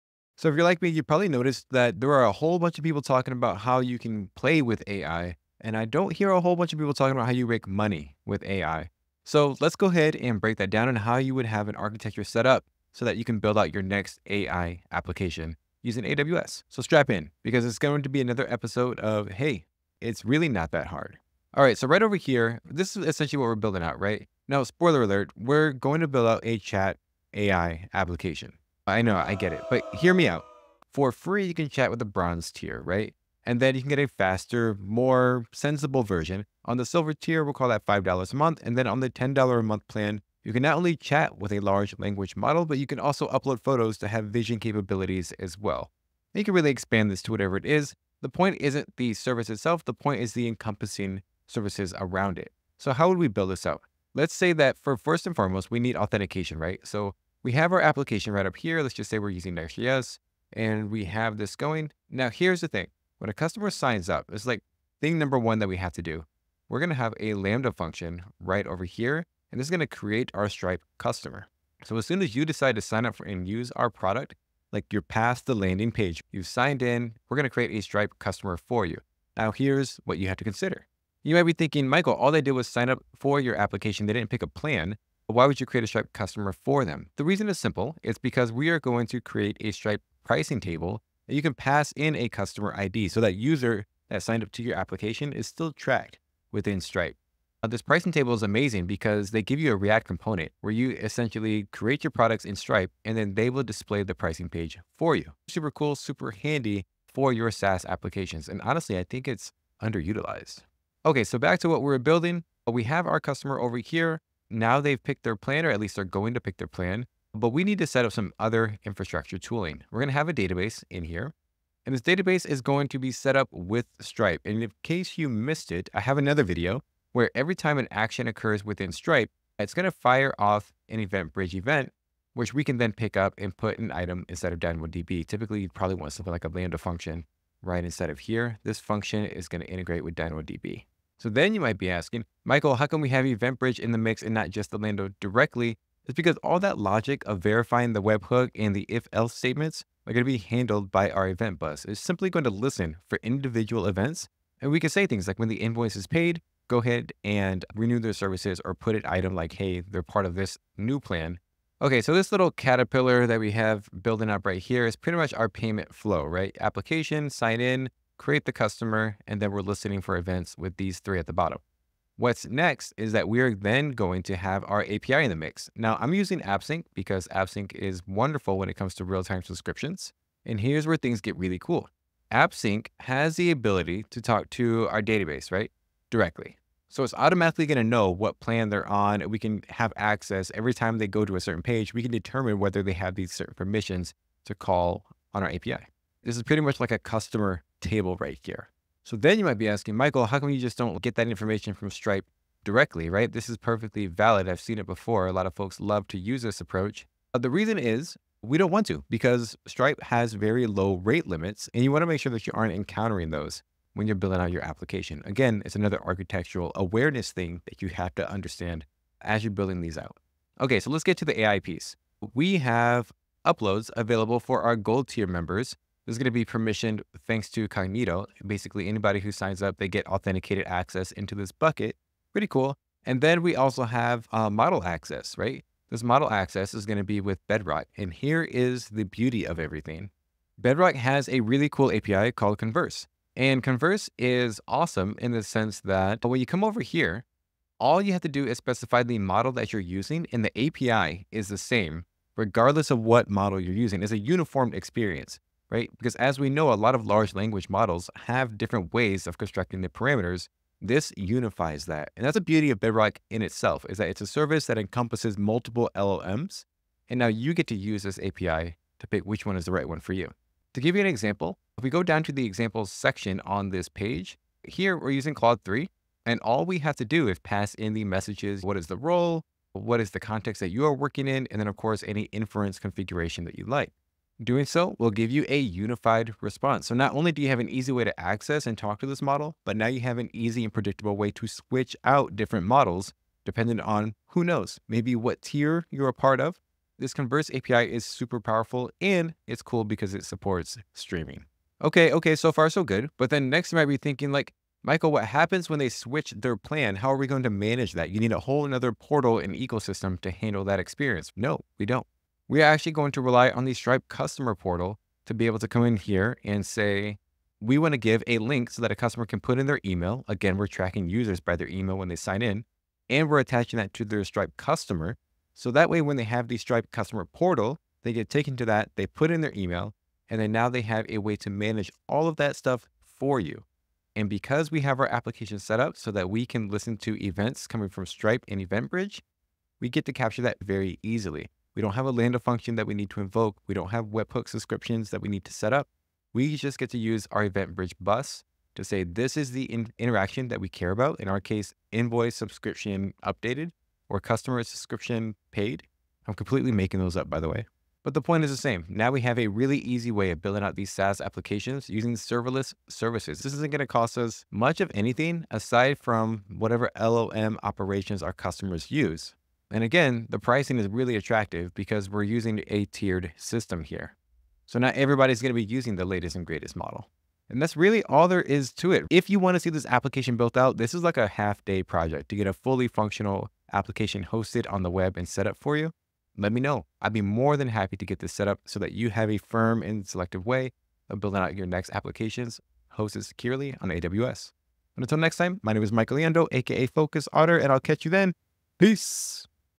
Architecture Bytes: Building an AI SaaS on AWS
let's combine what we've discussed in other videos to really showcase how to make money and generate passive income through the use of an AI SaaS!